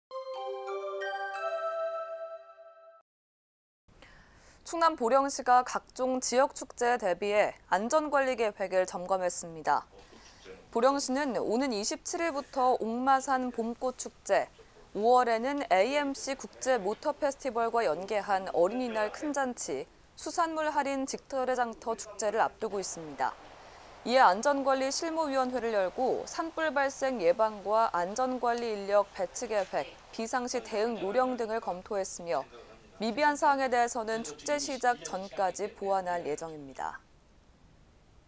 [대전뉴스] 충남 보령시, 지역축제 안전관리계획 점검
[아나운서]
충남 보령시가 각종 지역 축제에 대비해 안전관리계획을 점검했습니다.

[기사]
보령시는 오는 27일부터 옥마산봄꽃축제, 5월에는 AMC 국제모터 페스티벌과 연게한 어린이날 큰잔치, 수산물 할인 직거래 장터 축제를 앞두고 있습니다.

이에 안전관리 실무위원회를 열고 산불발생 예방과 안전관리인력 배치계획, 비상시 대응요령 등을 검토했으며 미비한 사항에 대해서는 축제 시작 전까지 보완할 예정입니다.